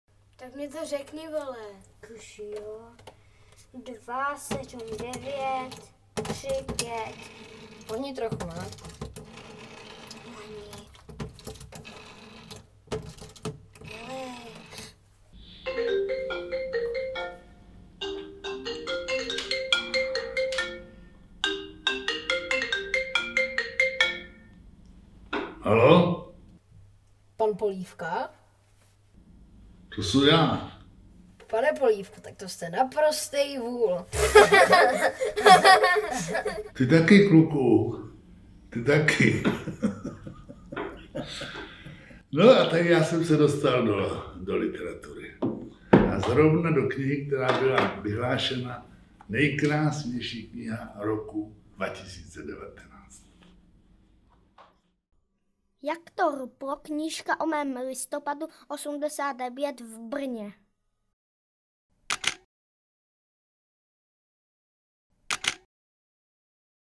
Kniha "Jak to ruplo"
Reklama na knihu Jak to ruplo pro TIC Brno, 2020

herci: Bolek Polívka, Jaromír Barin Tichý
dramaturgie a scénář: Michal Konečný
video a produkce: veryhotstudio

autor knihy: Michal Konečný
ilustrace: Barbora Straková
grafická úprava: Petr Škobrtal

"Originální pohled na události roku 1989 včetně těch listopadových nabízí nová knížka z produkce TIC BRNO s názvem Jak to ruplo. Je sice určena především dětem a mladším ročníkům, které sametovou revoluci nezažily, ale na své si přijdou také jejich rodiče a všichni ti, kteří uvítají neotřelý pohled na historické události neodpovídající tomu, co najdete v učebnicích."